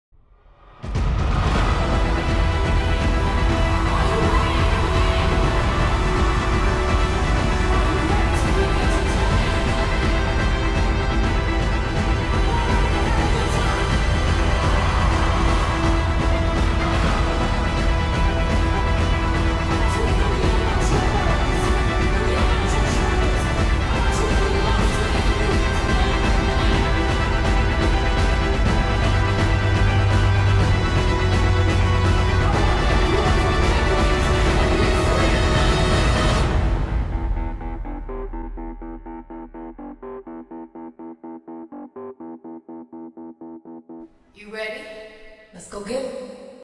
Countdown To Touchdown
Bey's performance at the Superbowl is coming closer and closer & so I made this little video for it and even though it's really short it took me many hours to make it so I really hope you enjoy it! :)